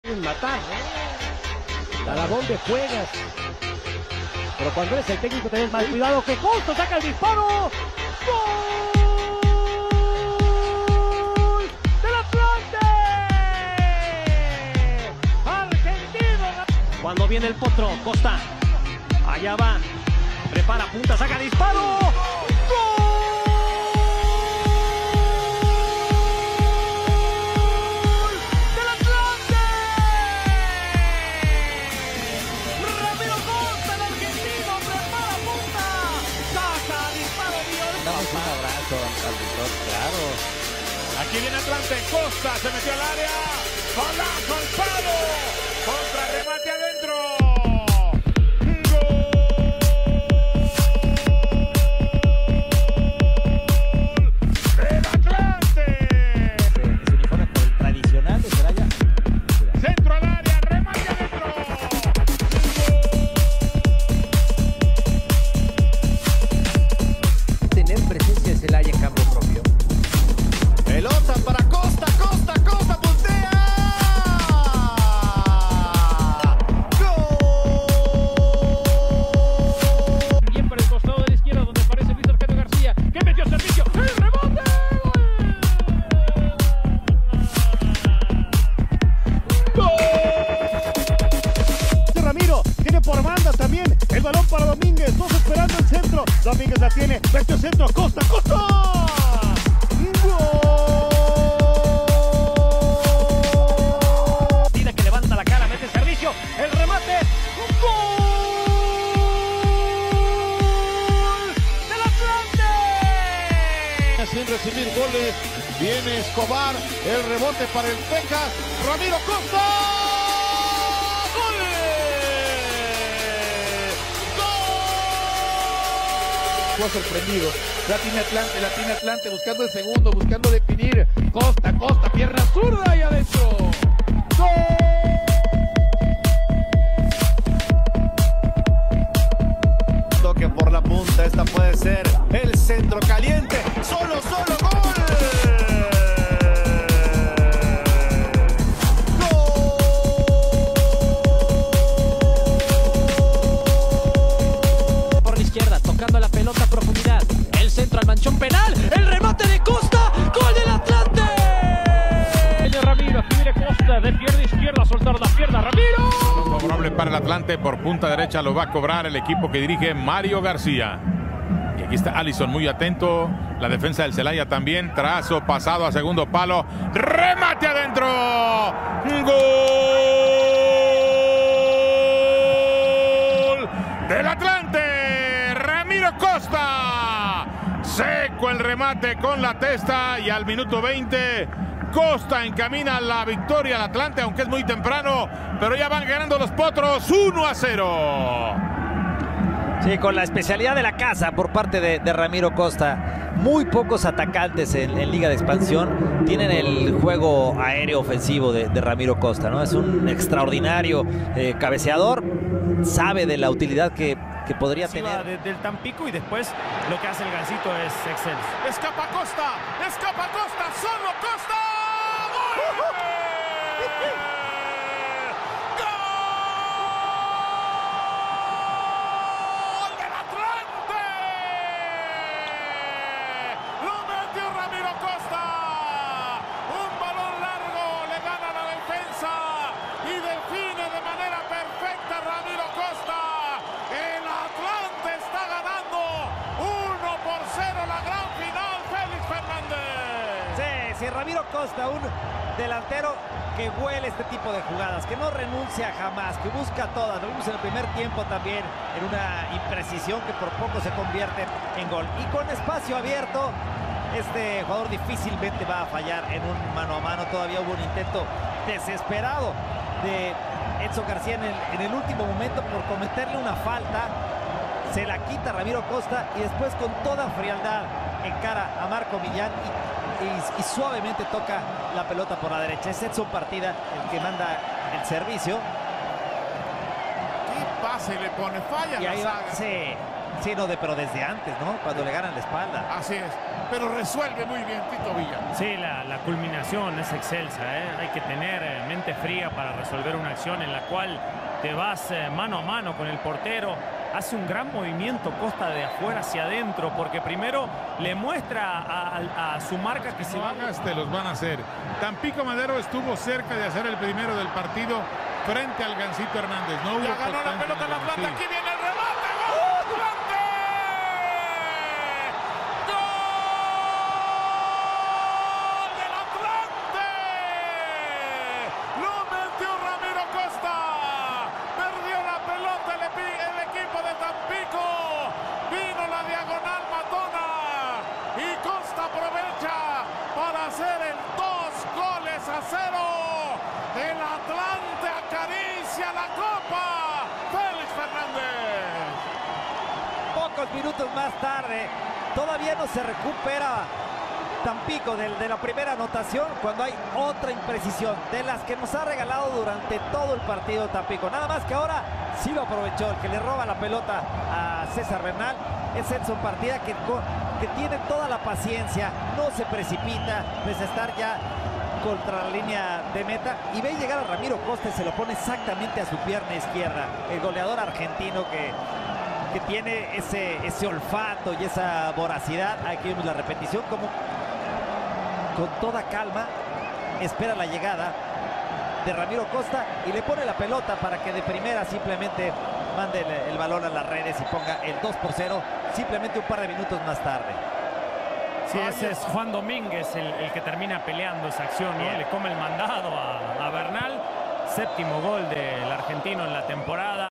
Ramiro Costa es nueva baja del Atlante 🔵🔴
Los potros de hierro se despiden de un gran delantero que sin duda dejo huella en el equipo.
Con sus múltiples anotaciones le dio muchas alegrías a los aficionados del potro, gracias a él se consiguió el primer campeonato del potro en la liga de expansión, sin duda es una baja muy sensible.
Gracias Vikingo por tantos momentos de alegría, suerte en tus futuros proyectos y recuerda que está siempre será tu casa.

Videos cortesía a quien corresponda